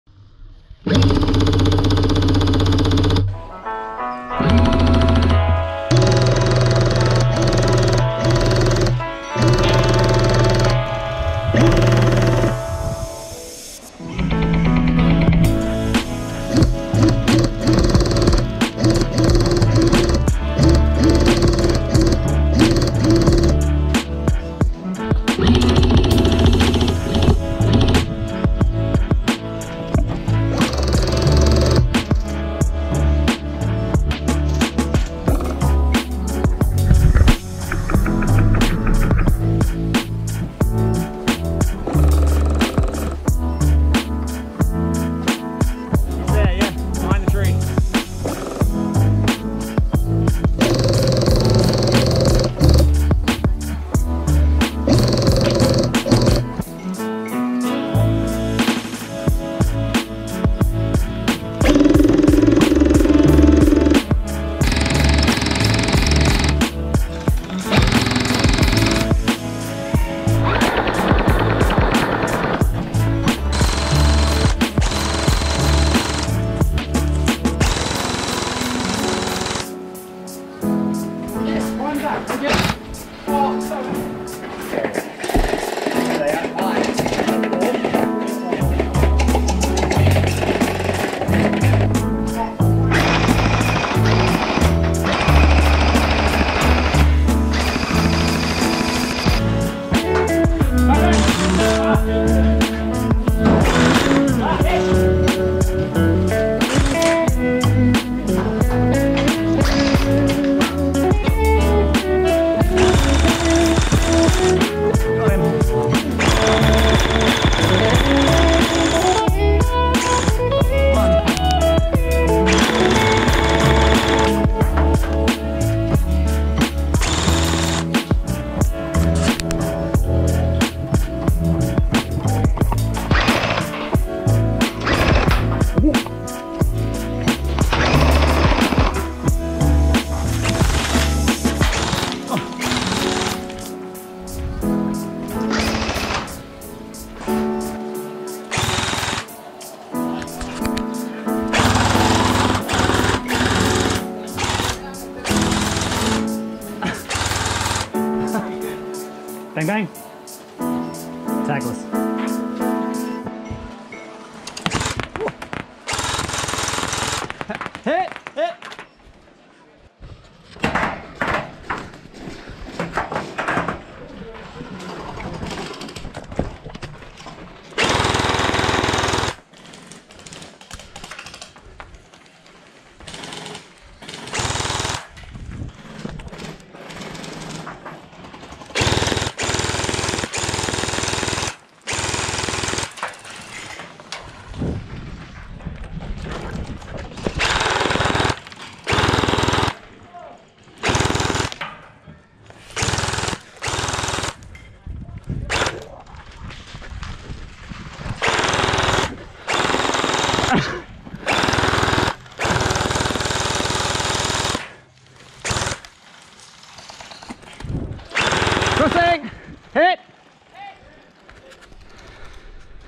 H2O accelerators
Just some random clips thrown together from some gel games recently.